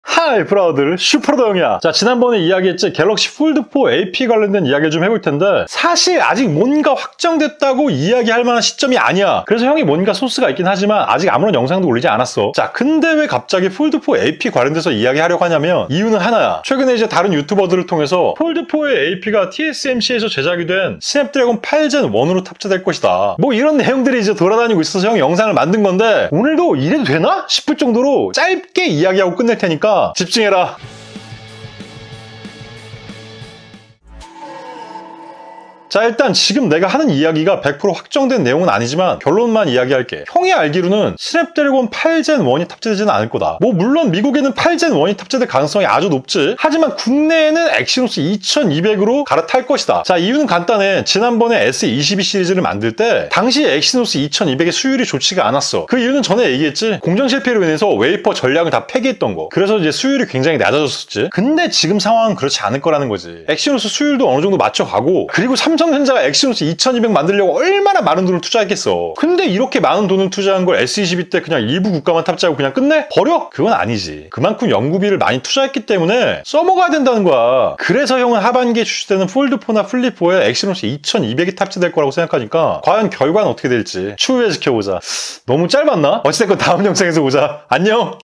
갤럭시폴드4 AP 스냅?엑시?
최근 슬슬 이야기가 나오고 있는 폴드4 관련 소식들
그 중 AP에 대해 이야기 해보았다.

Thumnail image : @letsgodigitalNL